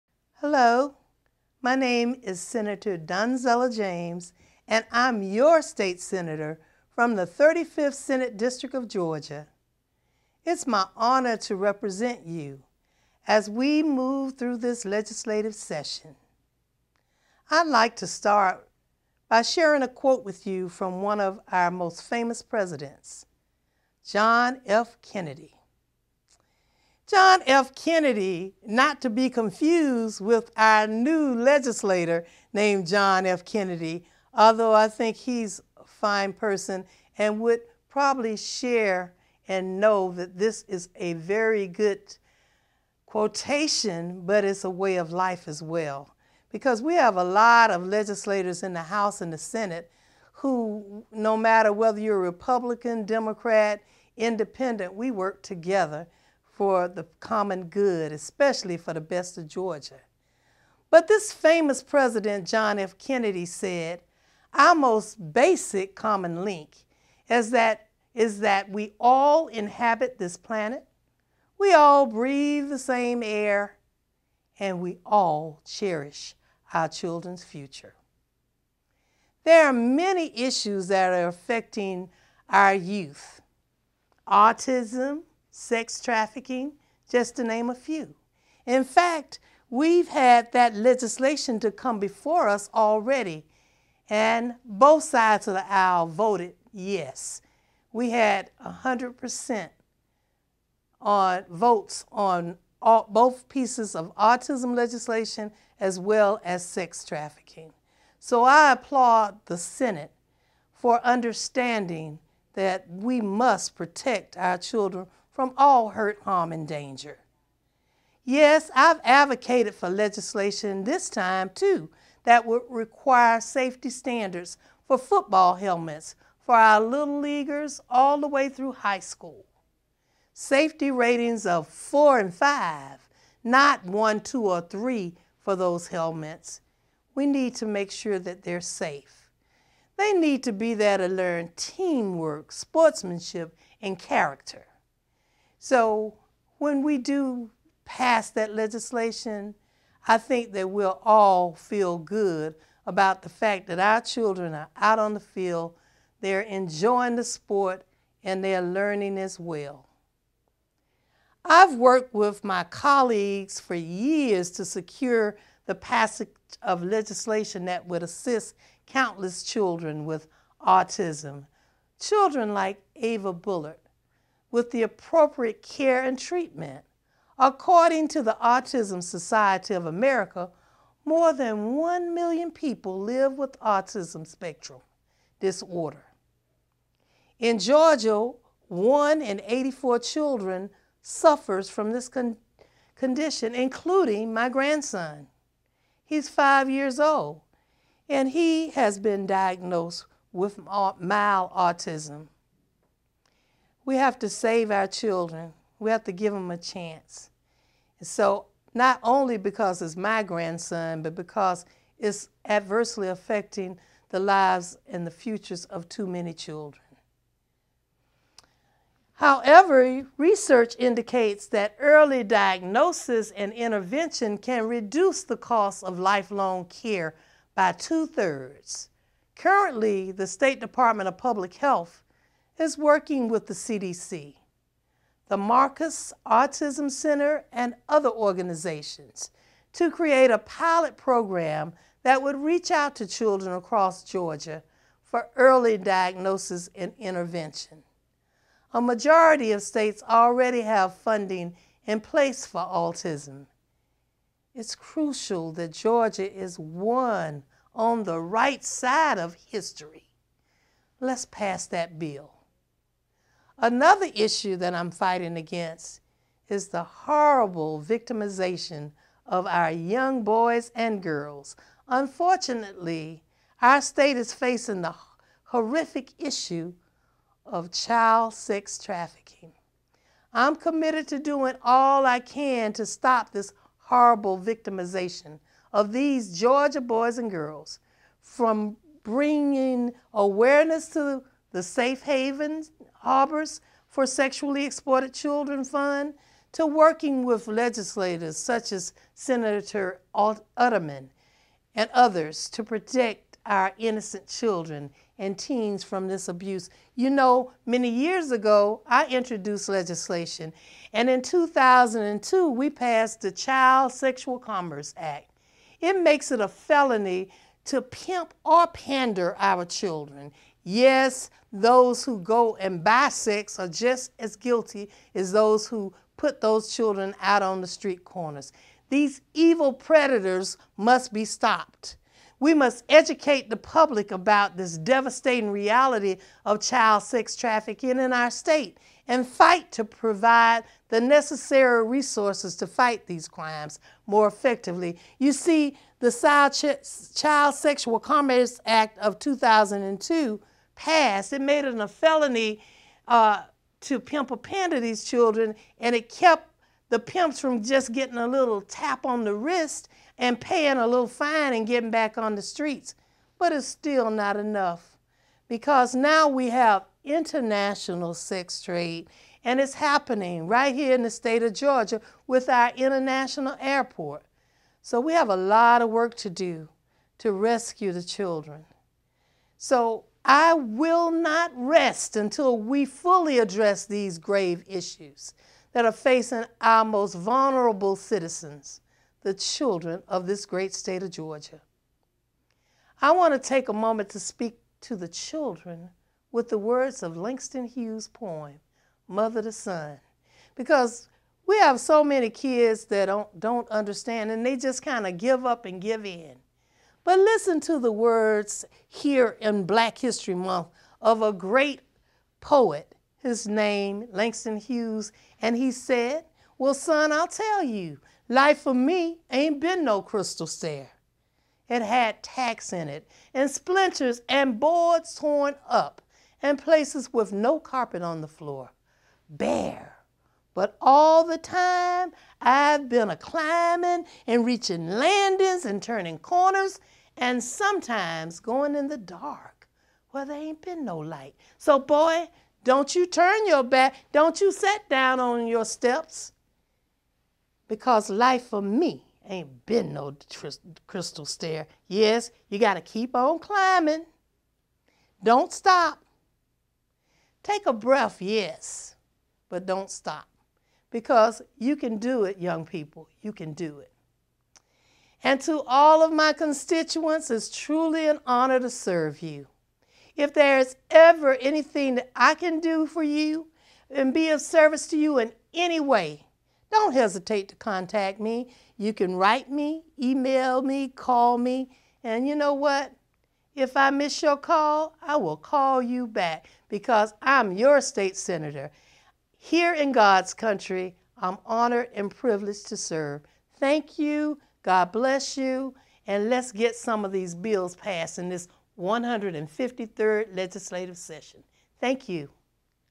Sen. James: Protecting Georgia's Children
Sen. Donzella James (D-Atlanta) talks about all the work the Georgia Senate is doing to protect Georgia's Children.